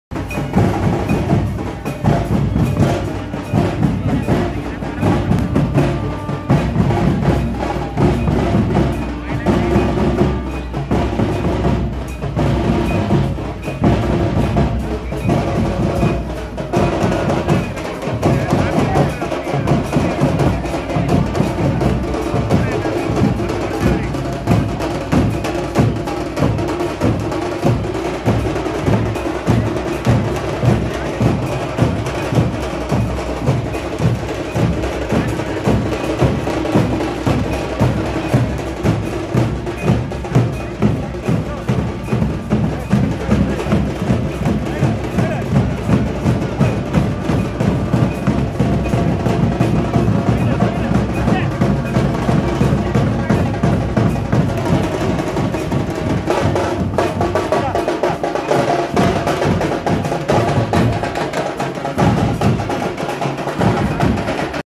Kollur Sri Mookambika🙏🙏🙏
2/4/2021
ಕೊಲ್ಲೂರು ಶ್ರೀ ದೇವಿಯ ಜಾತ್ರೆಯ ರಥೋತ್ಸವಕ್ಕೆ ದಿನಗಣನೆ
Kollur Sri Mookambika_AB
GROUP